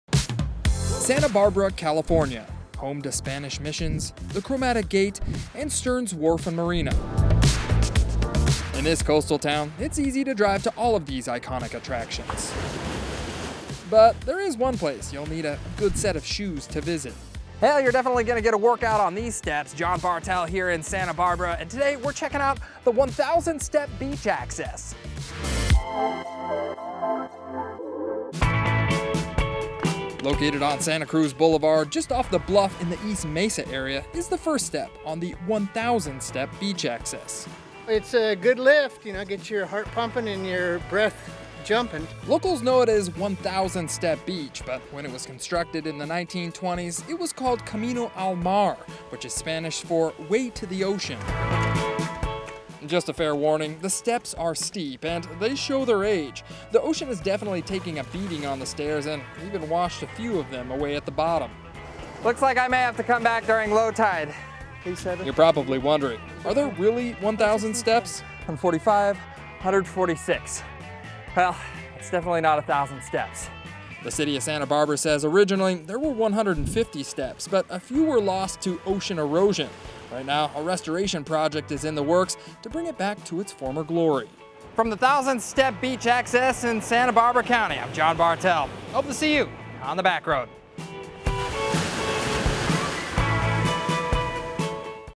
Santa Barbara's 'One Thousand Steps Beach Access' is a bit of a misnomer | Bartell's Backroads
It's not just the view that will leave you breathless. Find out how many steps it really takes to get to this Santa Barbara beach and why you should go.

Recorded pre-coronavirus so there are no masks or social distancing